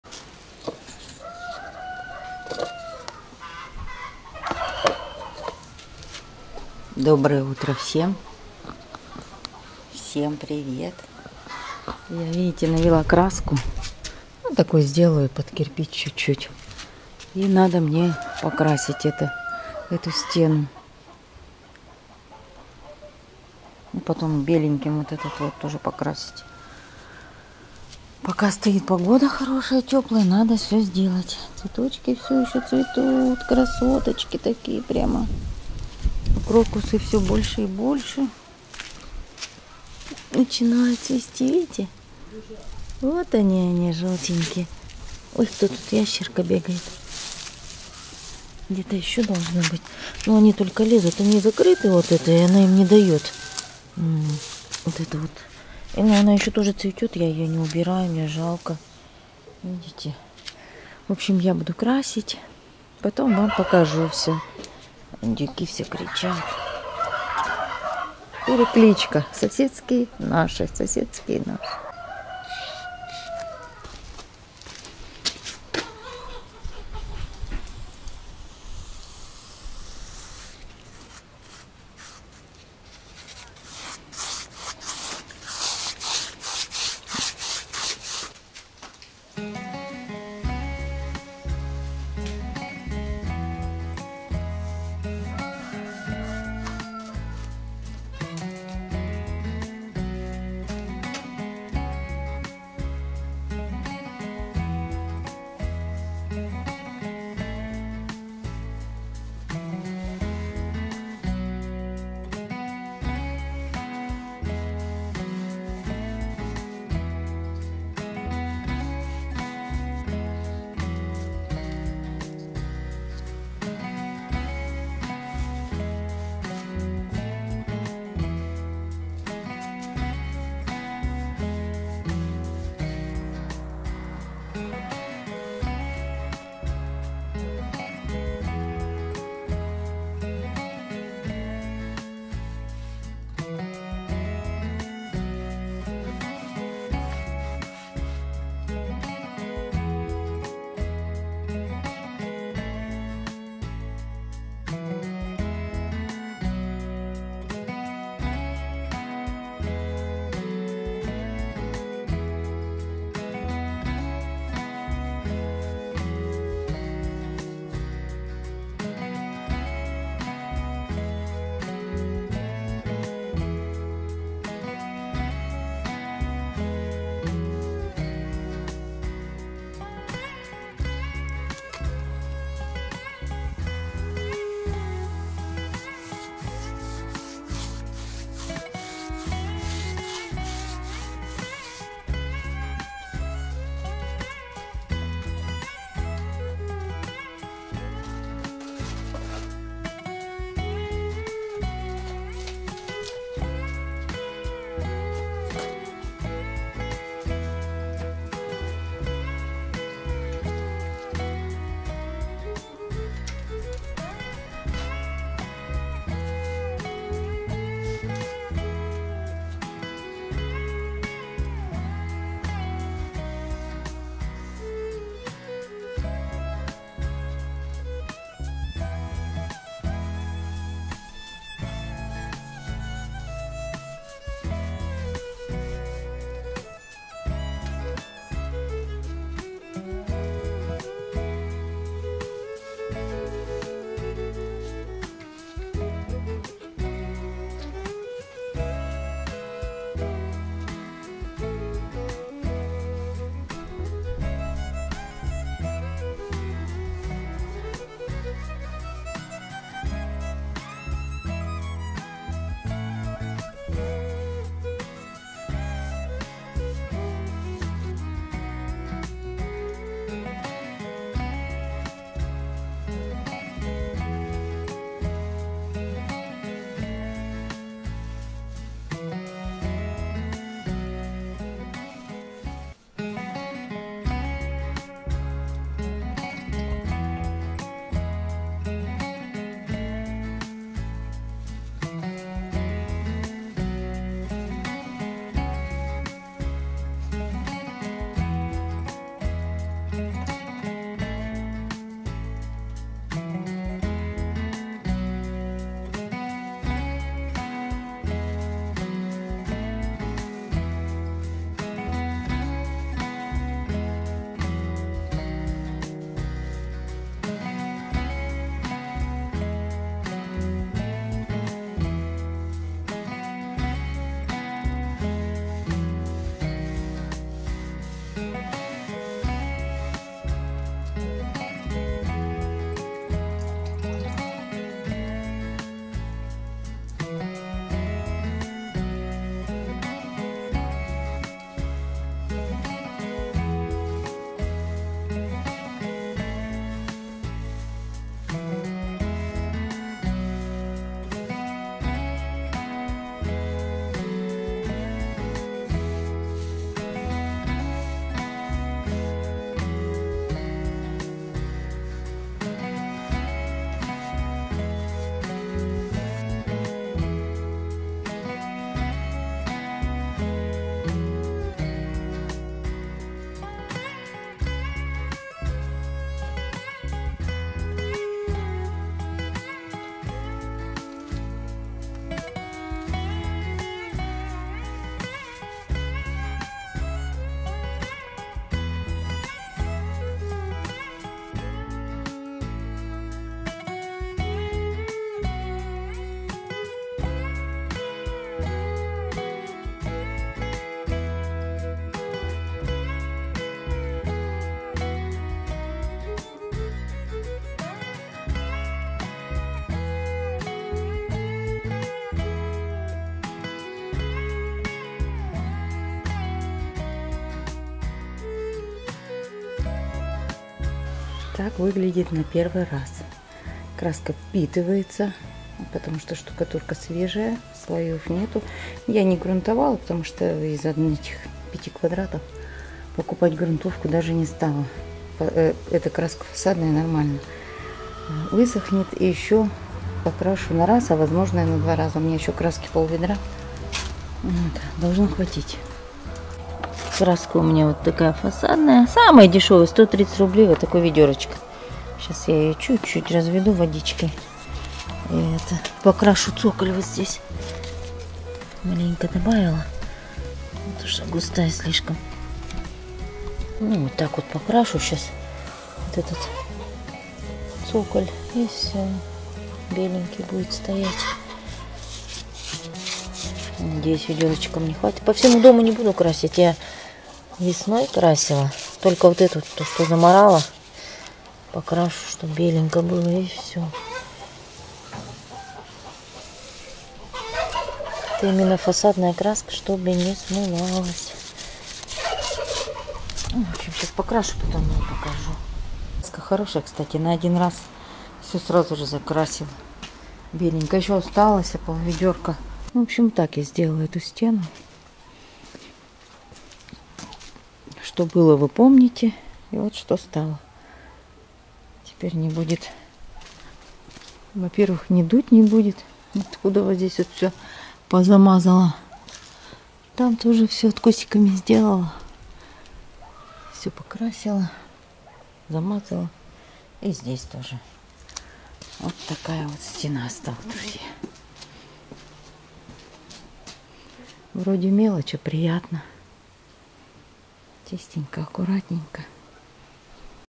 ПОКРАСИЛА СТЕНУ😊 МОЯ ДУША ДОВОЛЬНА 😋💃 WE LIVE IN THE KUBAN 😊🌺🌺🌺
ДРУЗЬЯ, ТЕПЕРЬ НОВЫЕ ВИДЕО СНАЧАЛА БУДУТ ПУБЛИКОВАТЬСЯ НА КАНАЛЕ ДЗЕН
Спасибо вам за поддержку!